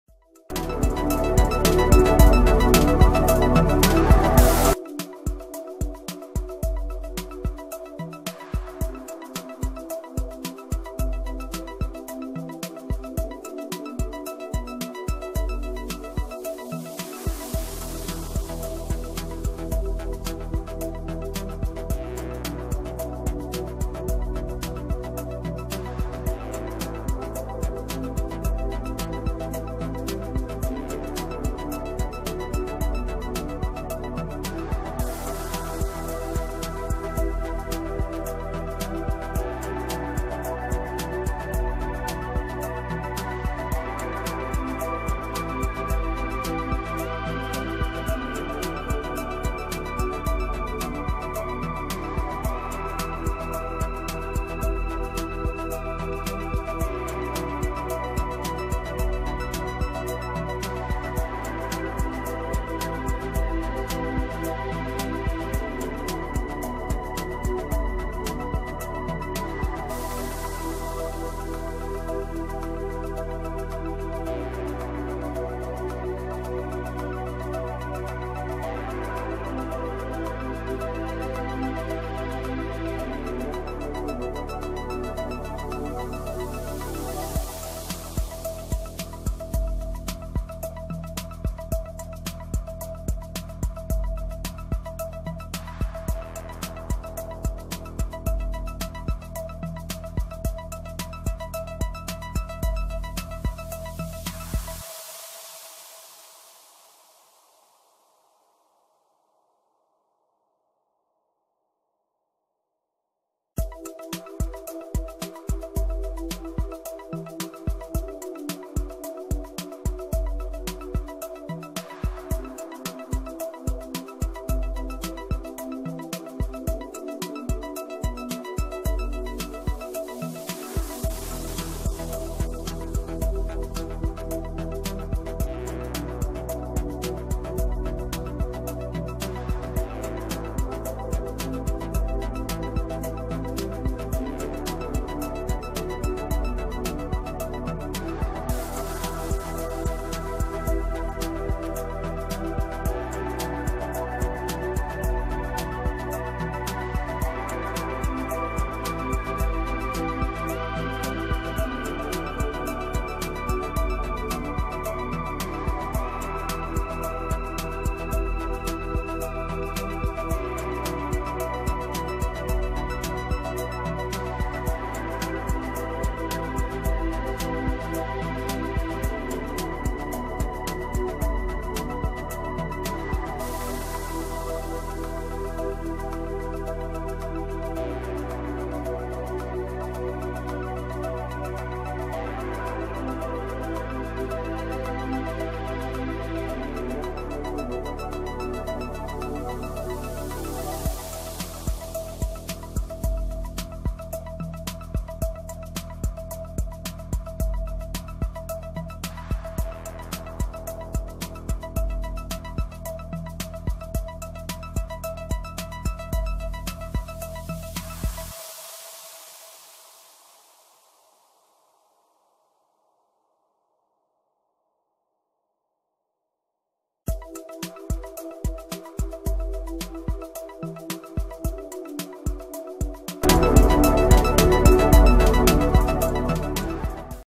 Comment regarder le statut whatsapp de quelqu'un sans qu'il le ne sache - خفية statut مشاهدة
Astuce simple et facile vous explique comment regarder le status whatsapp de quelqu'un sans qu'il le sache.

 طريقة مشاهدة statut بالواتساب دون أن يعلم صاحبها بذلك.